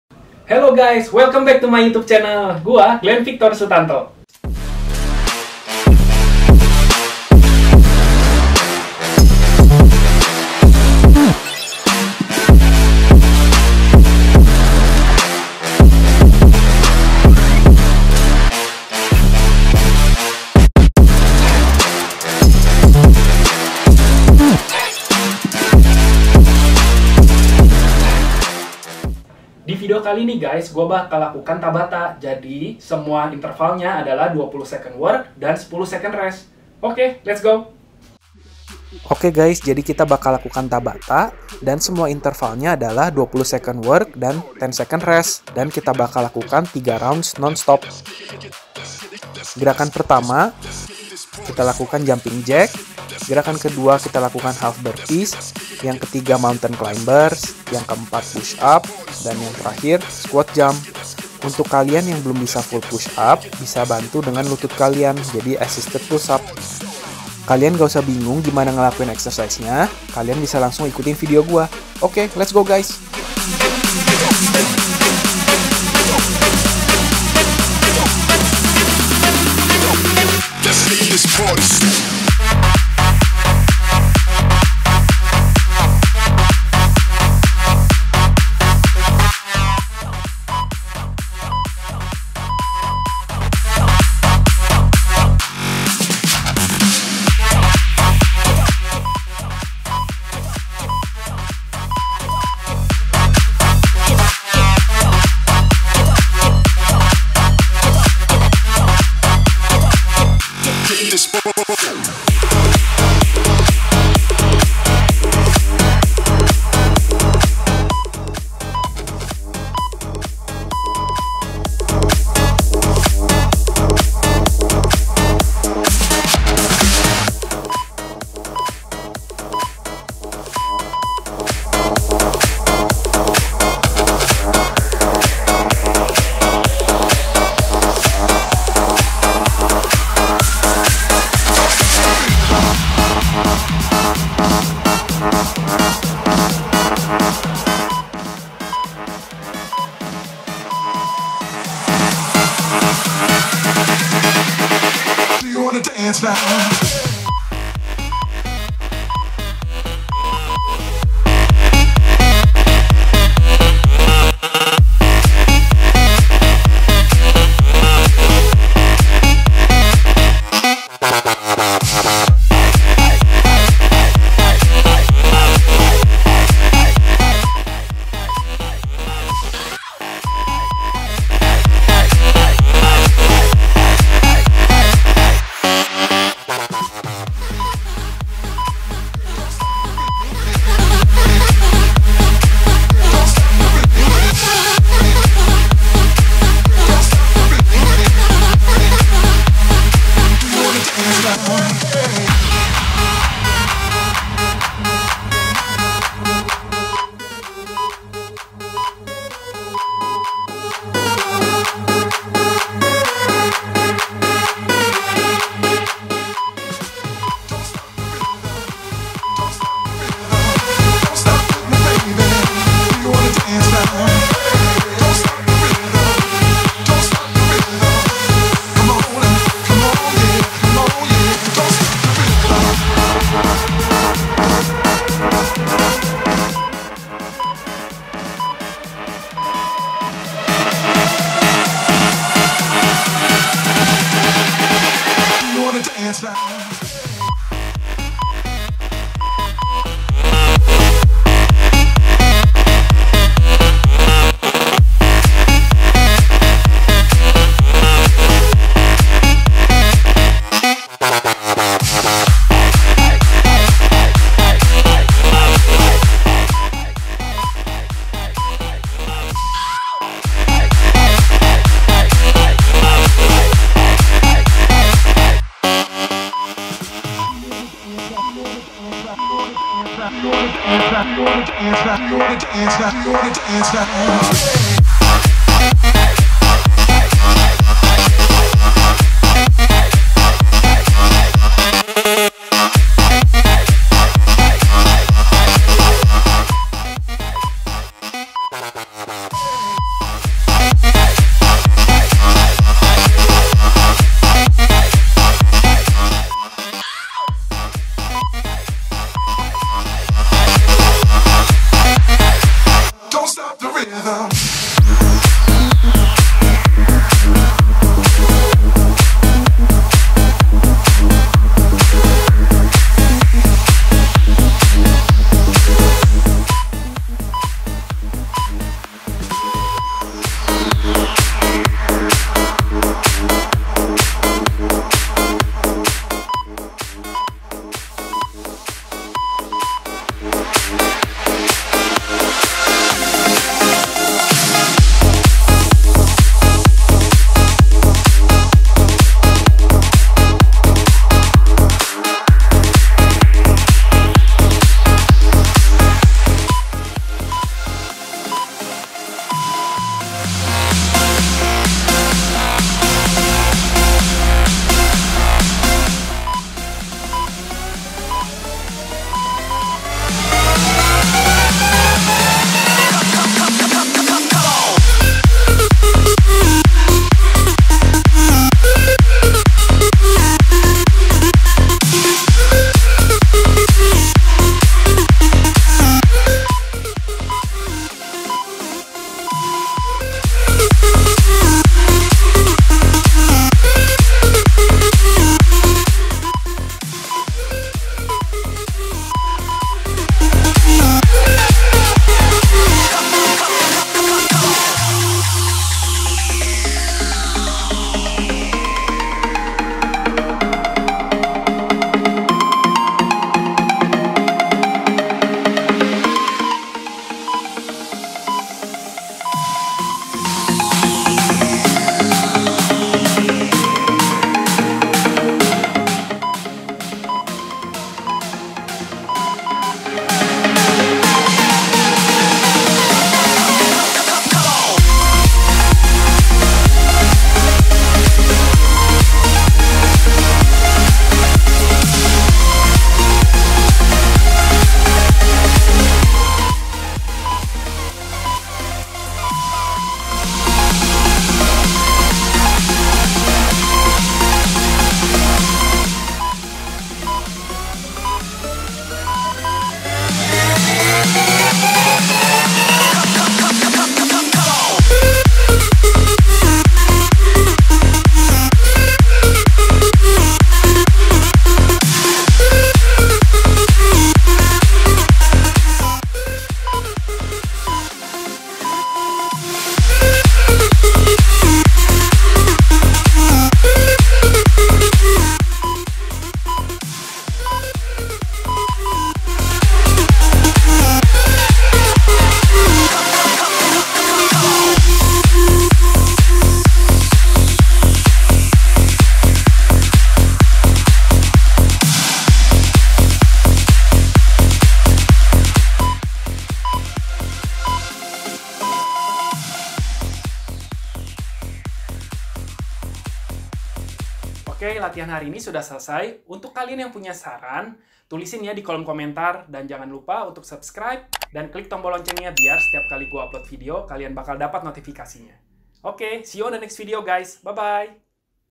BURN YOUR CALORIES WITH TABATA WORKOUT !!!! 🔥🔥🔥
Hi guys. Di video kali ini kita akan lakukan Tabata. Semua interval nya 20 work 10 rest. Kalian harus lakukan semua gerakan dengan intensitas yang tinggi. Asupan gizi pun harus diperhatikan yah guys biar hasil nya maksimal.

Workout :
1. Jumping jack
2. Half burpees
3. Mountain climbers
4. Push up
5. Squat jump